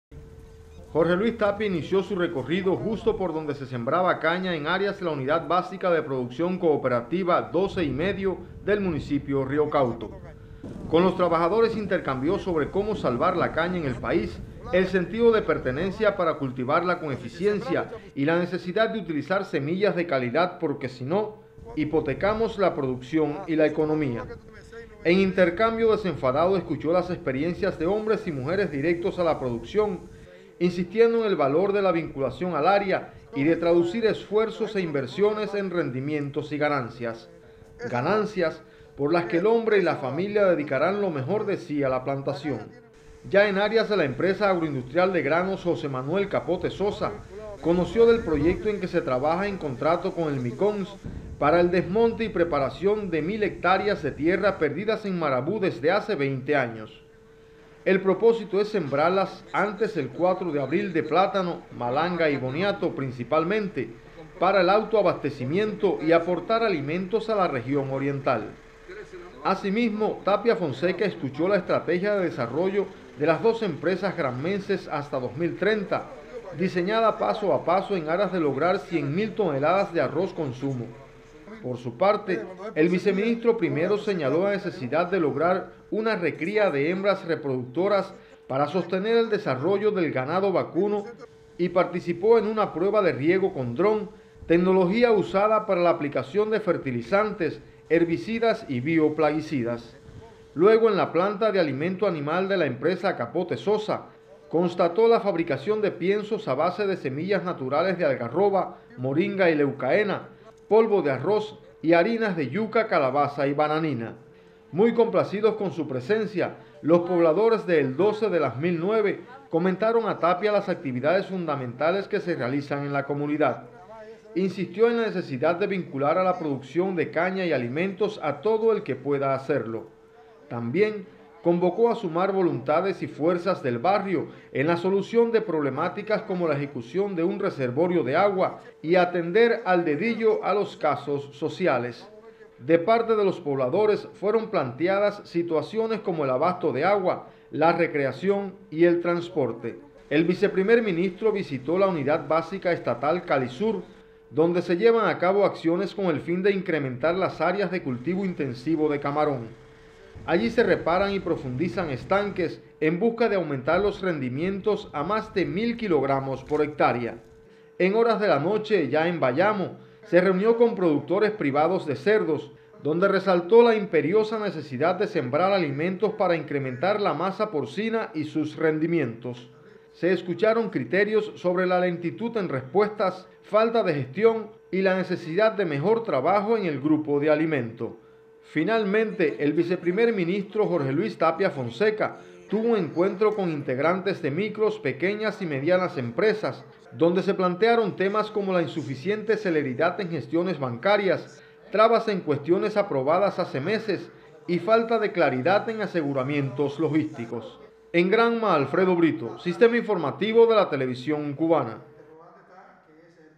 Recorre Vice primer ministro Jorge Luis Tapia Fonseca objetivos agrícolas y sociales en Granma
Como parte de  la visita gubernamental a la provincia de Granma el Vice primer ministro de la República de Cuba Jorge Luis Tapia Fonseca, recorrió entidades productoras de alimentos y comunidades rurales del municipio de Río Cauto y Bayamo. Con el fin de constatar la aplicación de las 63  medidas para potenciar el desarrollo de la agricultura, así como programas sociales  e intercambiar con productores y nuevos actores económicos, el alto dirigente cumplió una larga jornada de trabajo